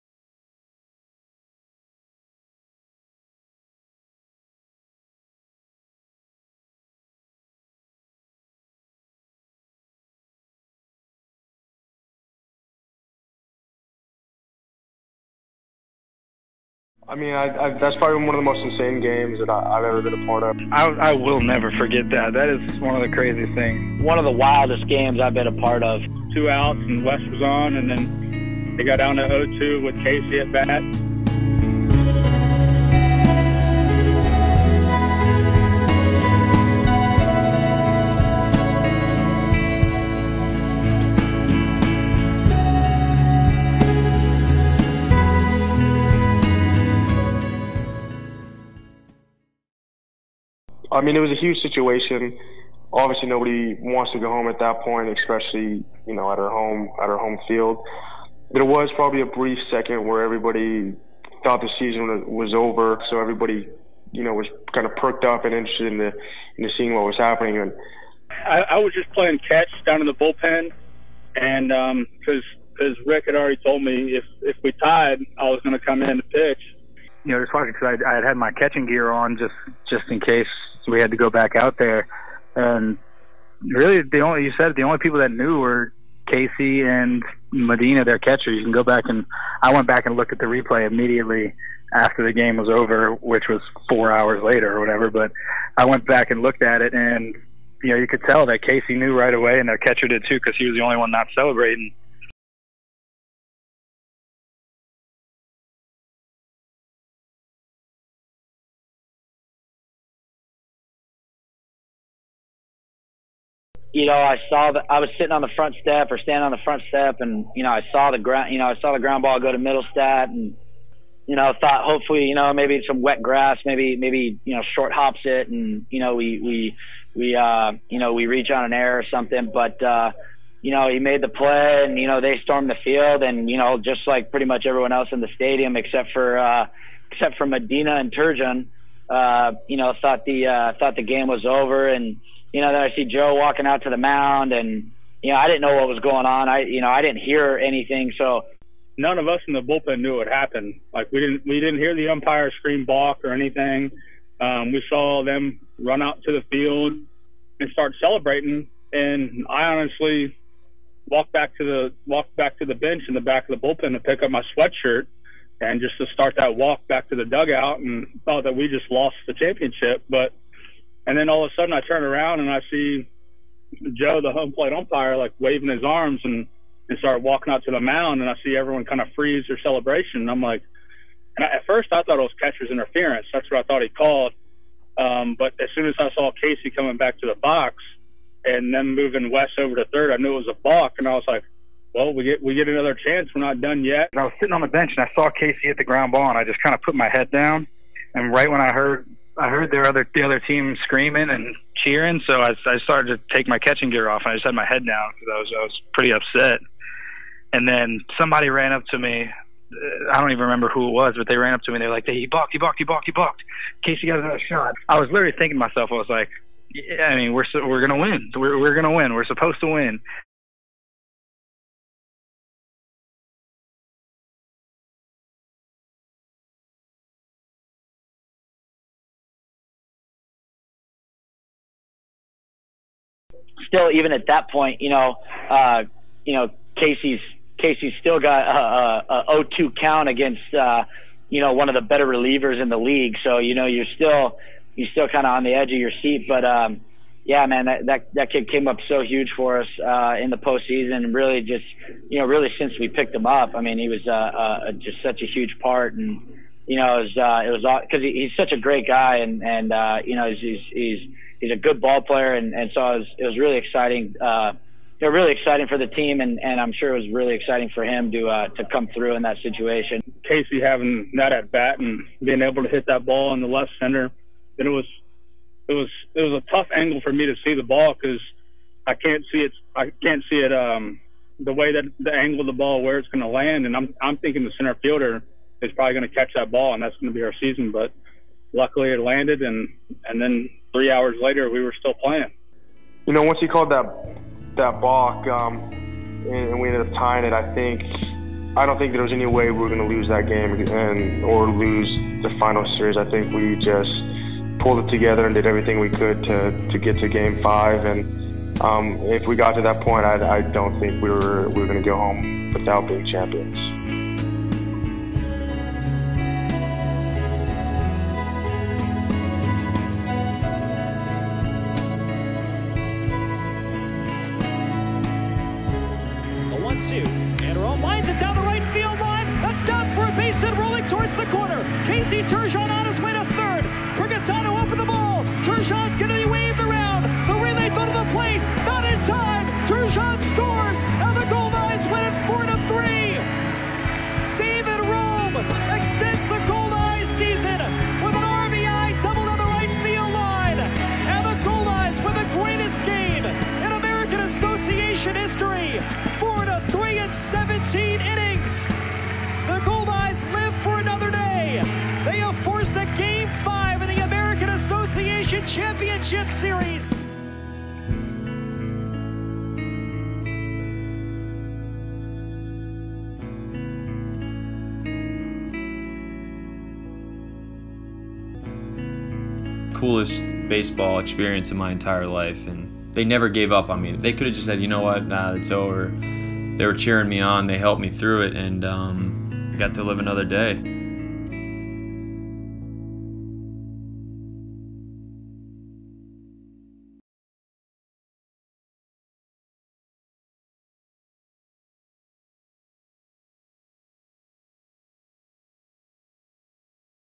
Casey at Bat
Game four of the 2017 American Association Championship series will go down in history. Casey Turgeon gave the Goldeyes life after a balk was called against the Wichita Wingnuts in the bottom of the ninth.